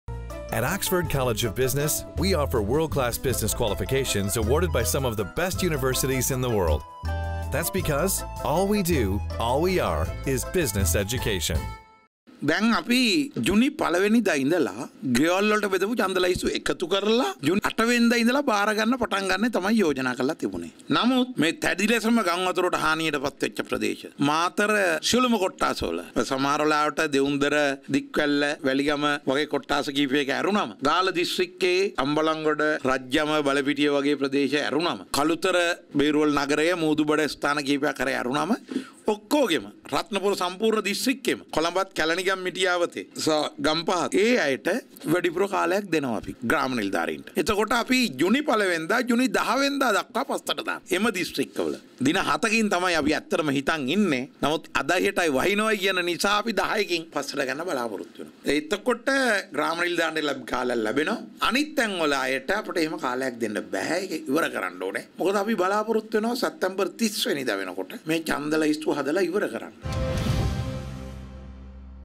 Collection of several Voters Lists delayed
කොට්ඨාශ කිහිපයක ඡන්ද ලැයිස්තු එකතු කිරීම ප්‍රමාද කෙරේ...